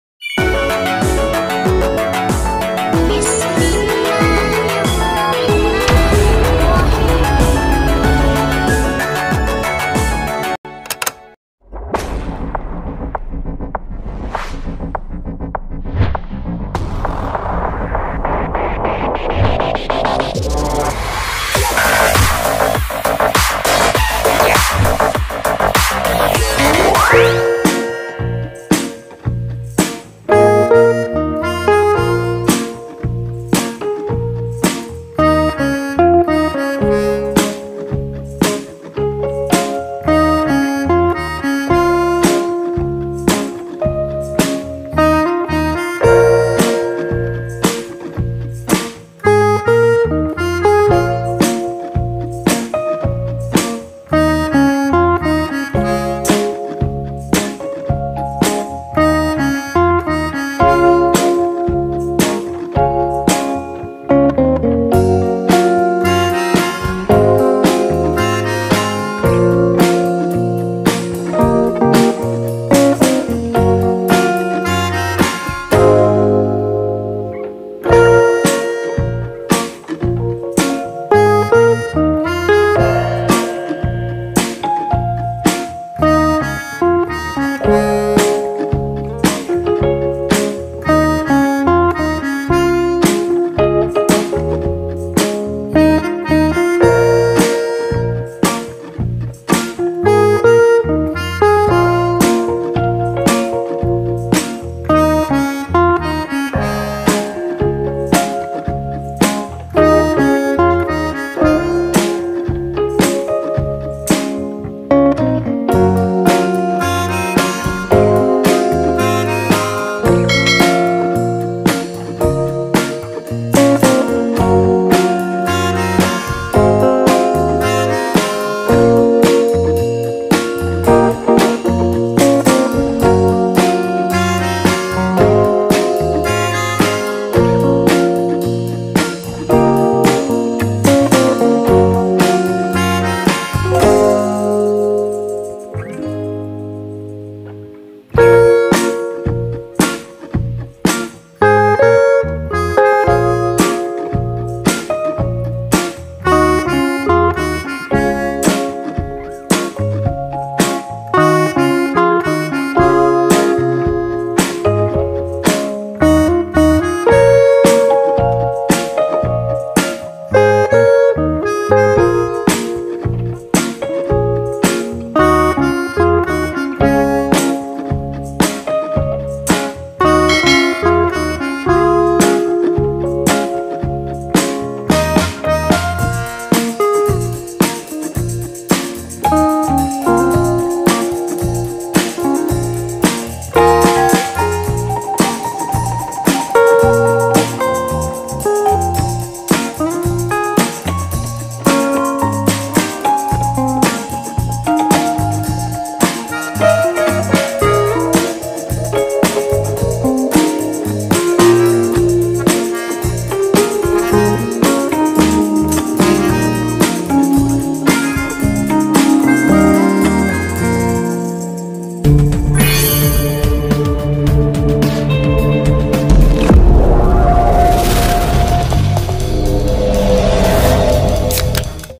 "CARA PANGKAS LAGU YANG DI KLAIM"
Live Kena KHC diujung, iklan diawal, pangkas deh daripada berbagi 🤭

Terimakasih Sobat sudah meluangkan waktu menyimak Video ini, Semoga Bermanfaat & Menginspirasi